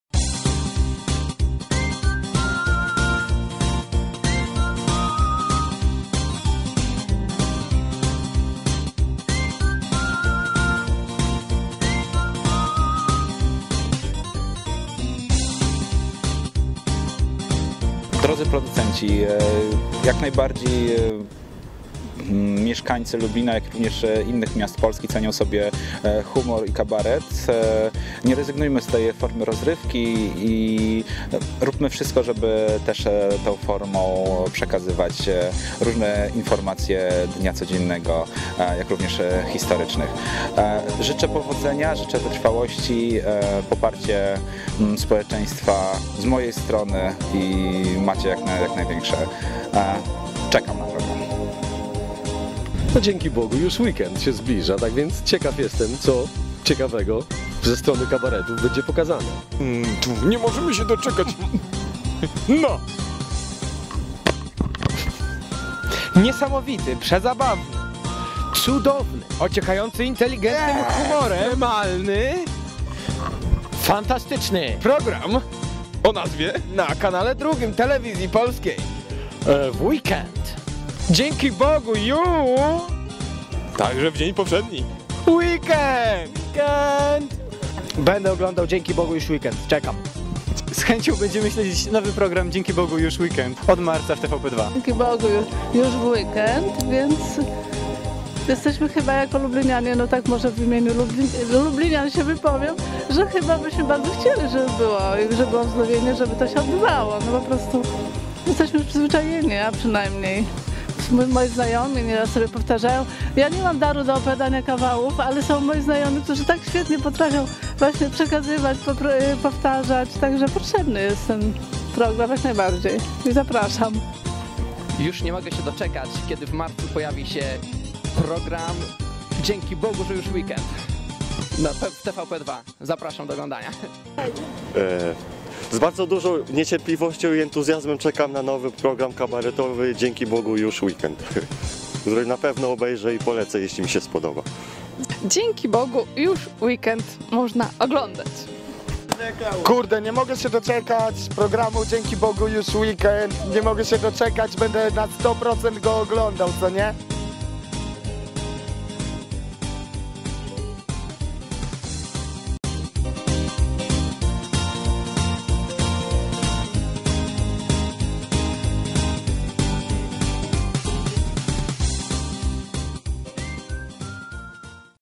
Projekt Kabaret: Dzięki Bogu już weekend- od 1 marca, TVP2, 21:45!
W TVP2 rusza właśnie nowy program kabaretowy o wyjątkowo nietypowej formule. "Dzięki Bogu już weekend" będzie bowiem emitowany co tydzień na żywo! W roli prowadzących poszczególnych odcinków zobaczymy m.in. Kabaret Moralnego Niepokoju, Kabaret Nowaki, Łowców.B, Kabaret Smile, Kabaret Młodych Panów i wielu innych! Dodatkową atrakcją będzie program "Tylko dla dorosłych" prowadzony przez Kacpra Rucińskiego, w którym widzowie będą mogli zobaczyć różne oblicza stand up'u i nie tylko. Lublin będzie obecny przed telewizorem, a Ty? Zobaczcie, co widzowie opowiedzieli nam w sondzie ulicznej.

(w filmik wdarł się błąd, program będzie emitowany o 21:45)